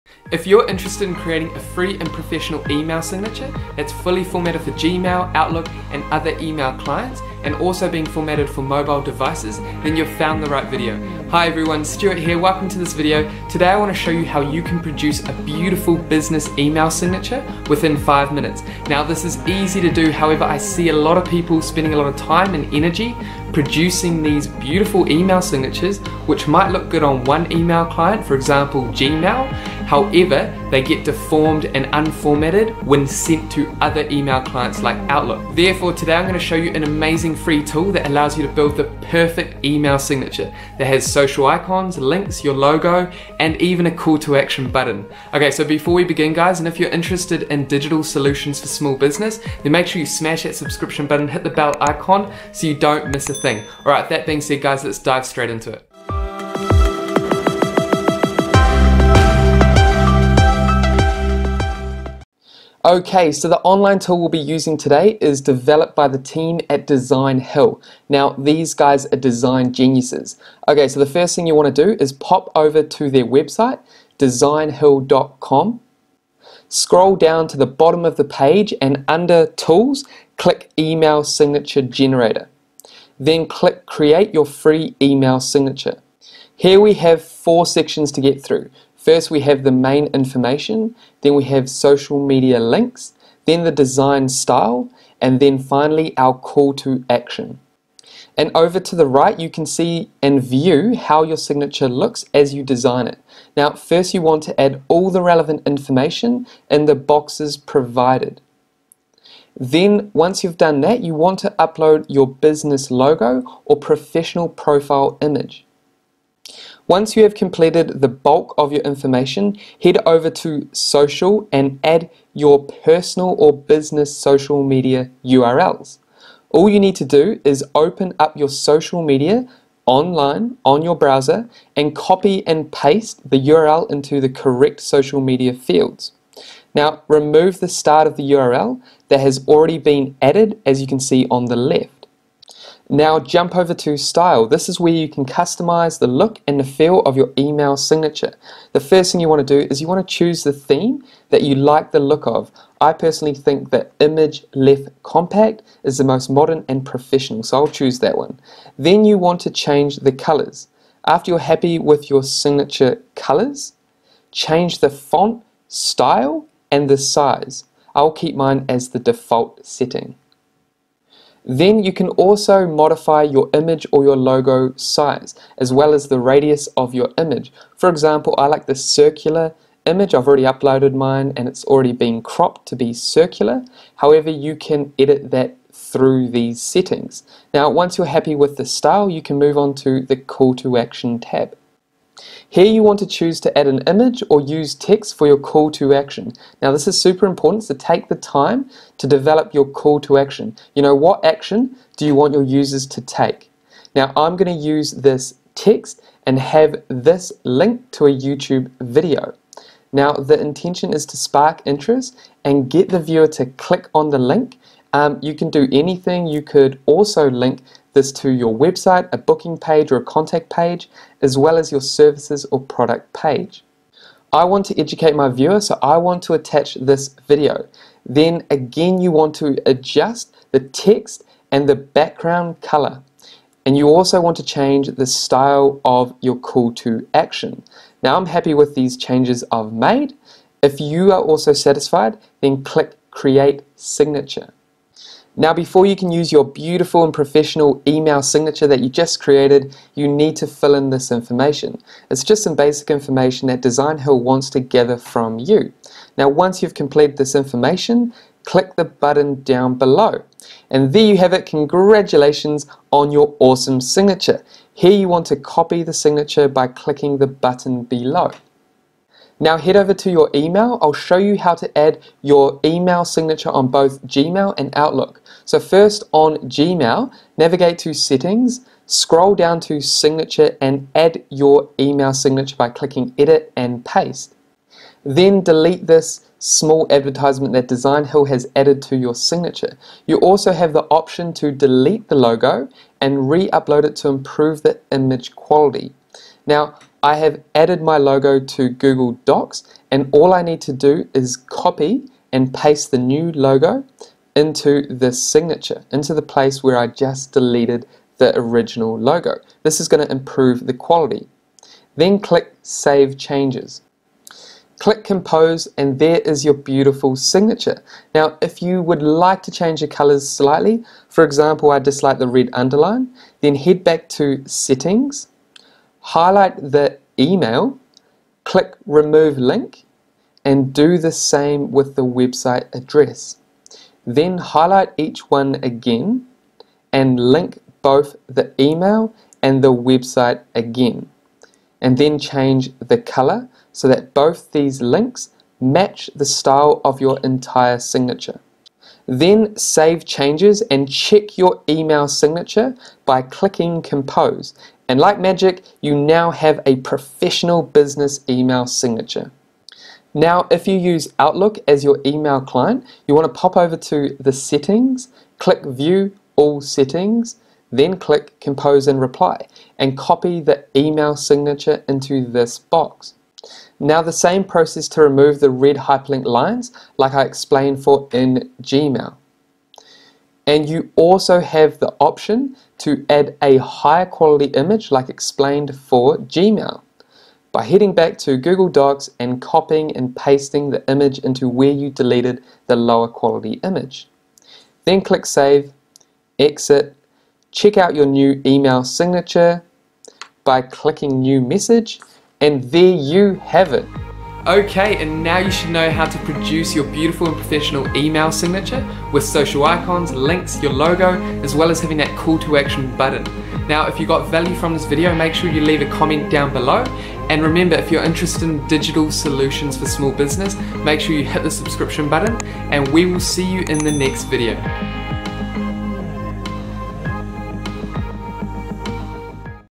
How To Create A Professional Email Signature - Logo, Social Icons & CTA links (Outlook + Gmail) 2020
In this video, I show you how to create a fully formatted and professional email signature for Gmail, Outlook and other email clients.

Designhill has amazing email signature templates that you can customise the way you like without knowing any code for all those formatting issues many people face with when creating their professional email signatures.

This is not just a basic email signature but one that allows you to add your Logo, Social Icons, links and a Call to Action button.